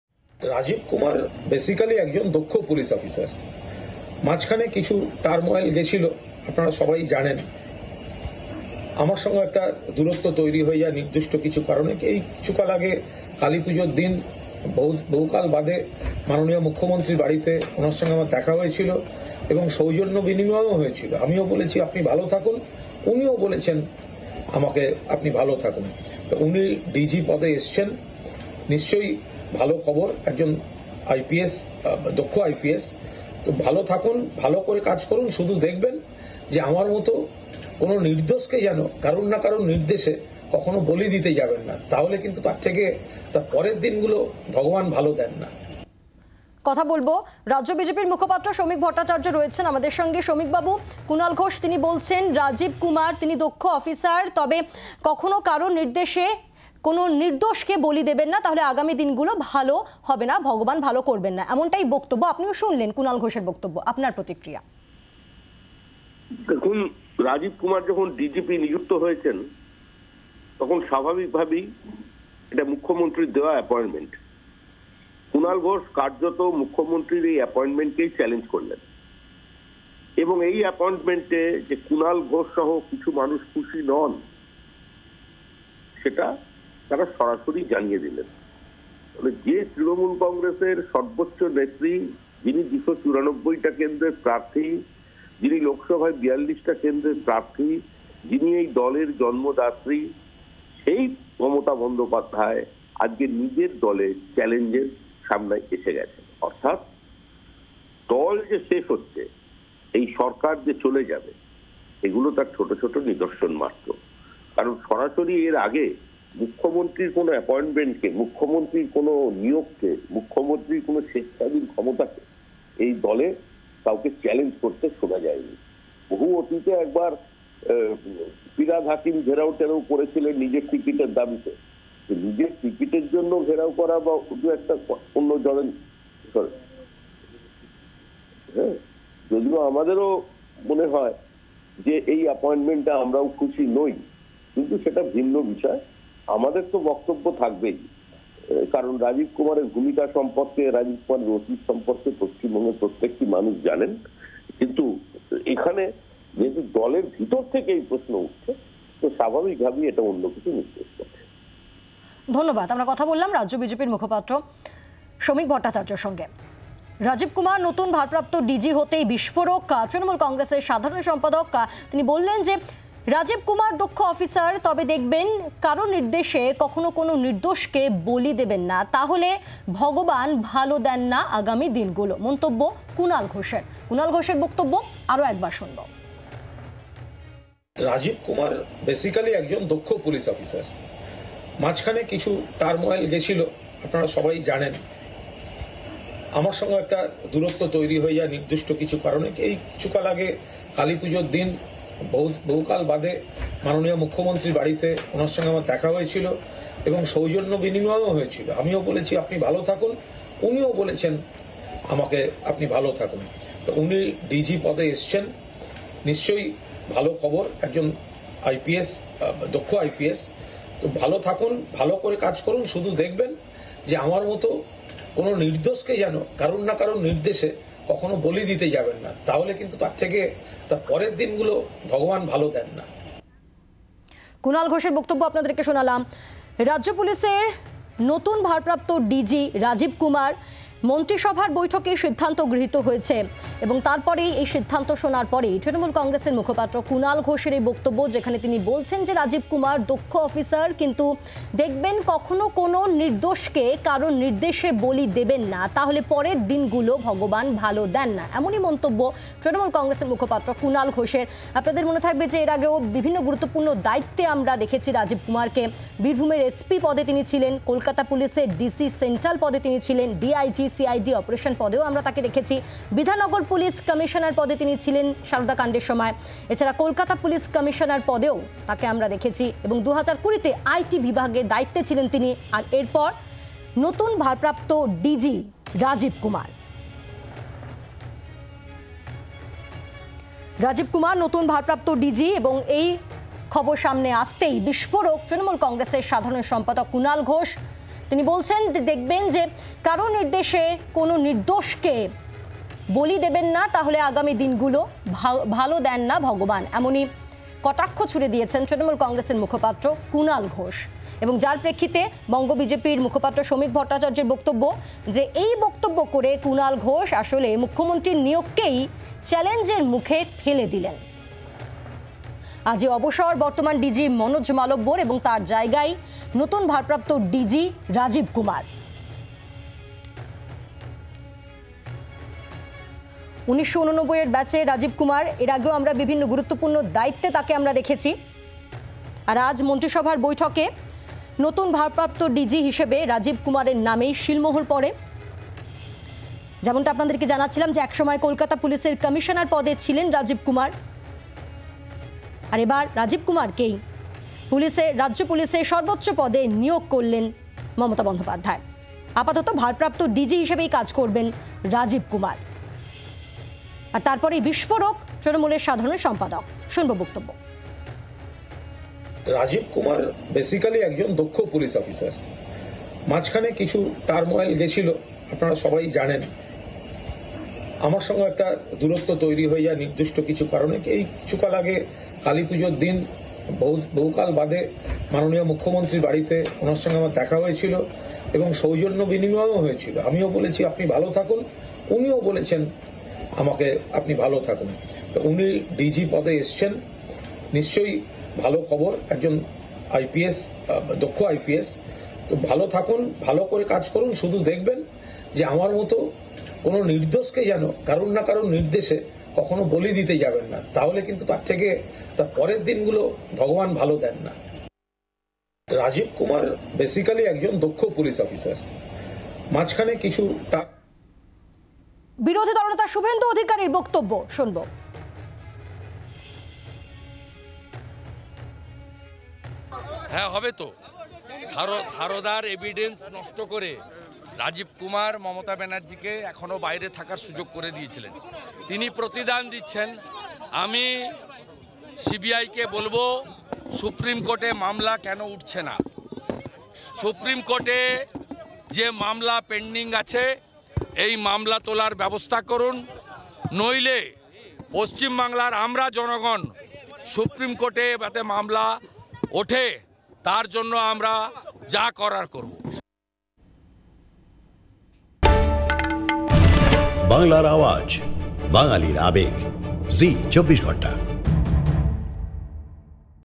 Kunal on DG: কুণাল ঘোষের ডিজিকে নিয়ে বিস্ফোরক মন্তব্যের পাল্টা কটাক্ষ শমীকের! | Zee 24 Ghanta
Kunal on DG: কুণাল ঘোষের ডিজিকে নিয়ে বিস্ফোরক মন্তব্যের পাল্টা কটাক্ষ শমীকের! দেখুন রাজ্যের নতুন ডিজি রাজীব কুমারকে উদ্দেশ্য করে তৃণমূলের রাজ্য সাধারণ সম্পাদক কী মন্তব্য করেছেন আর এর পাল্টা কী বলছেন বিজেপির মুখপাত্র শমীক ভট্টাচার্য... | Zee 24 Ghanta